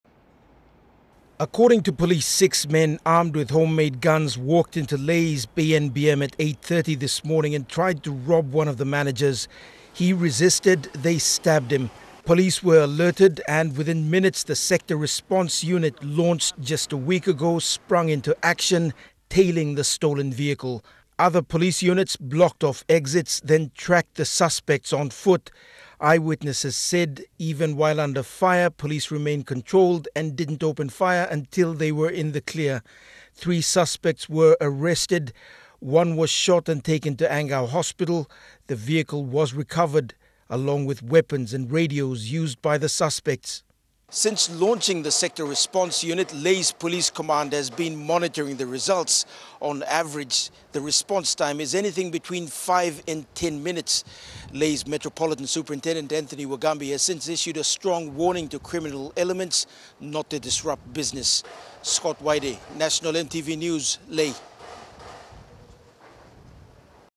Lae Police Sector Response Unit Arrests Armed Criminals
Just days after it was launched, the Lae Police Sector Response Unit has  begun netting in armed criminals.

This morning,  the unit responded within minutes of an armed robbery.

Three suspects were caught. At least one was taken to hospital with  gunshot wounds.